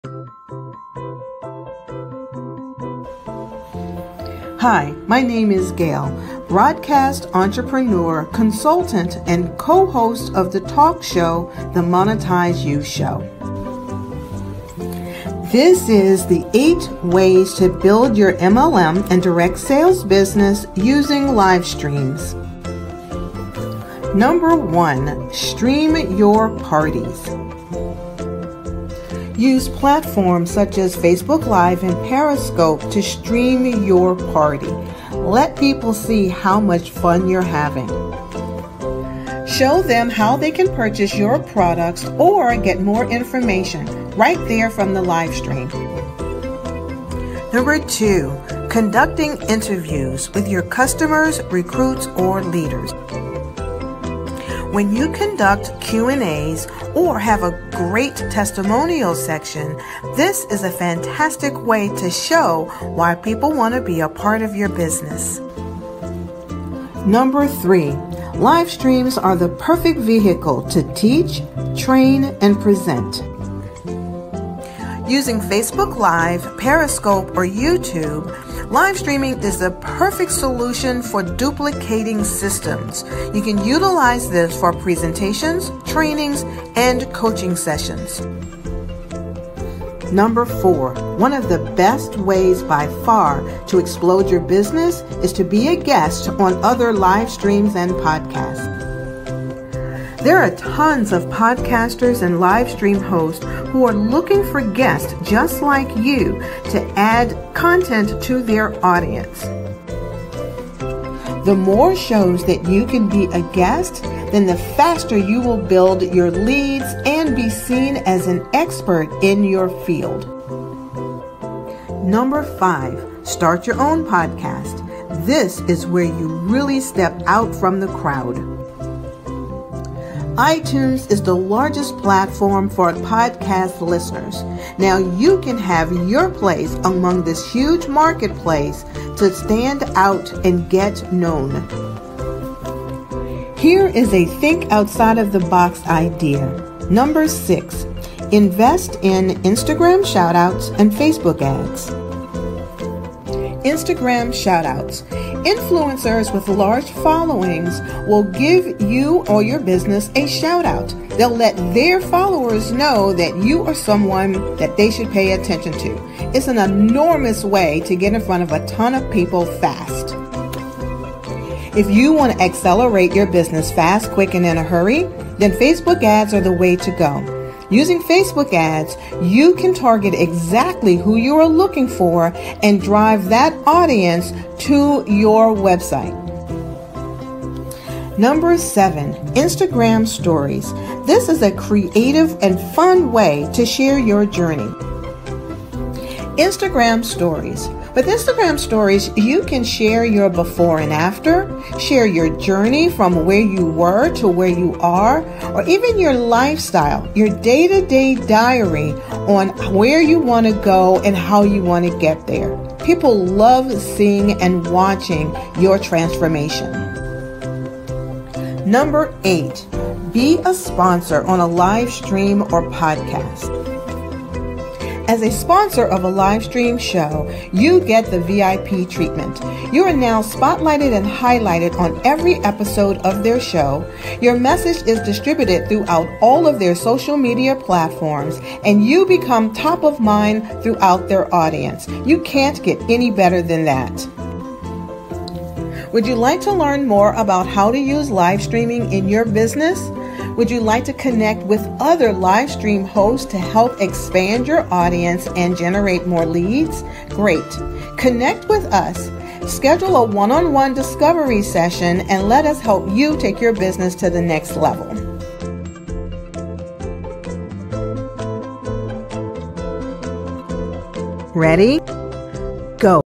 8 Ways to Build Your MLM Business Using Livestreams
Discover ways to use livestream technology to build and expand your leads, customers and partners.

1. Home Parties
2. Conduct Interviews
3. Train, teach Present
4. Become a guest on shows
5. Start a Podcast
6. Invest in IG Shoutouts and FB Ads
7. Use IG Stories

Get more ideas and strategies. Connect with show hosts and podcasters.  Schedule a discovery session with us at
Bit.ly/TalktoGail